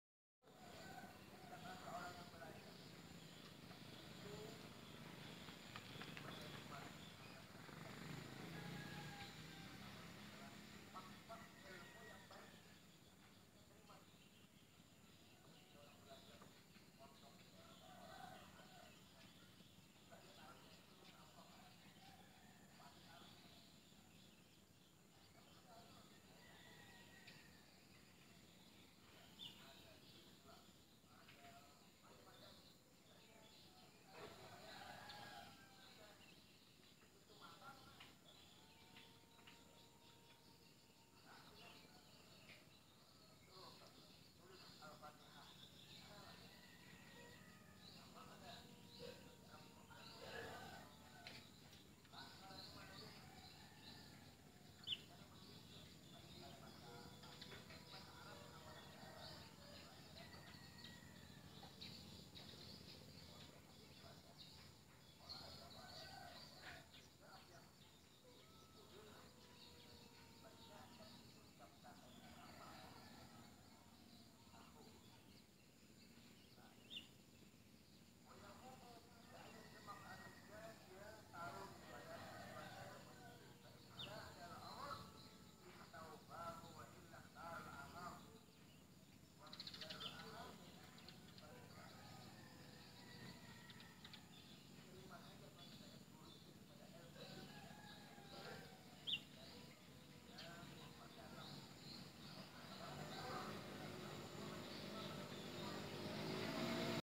Jet melintas di atas Kuala Kapuas
INFORMASI KAPUAS - Video ini direkam saat berada di Jalan Kapten Pierre Tendean, Kuala Kapuas, Kalteng pada hari Jum'at, 17 November 2017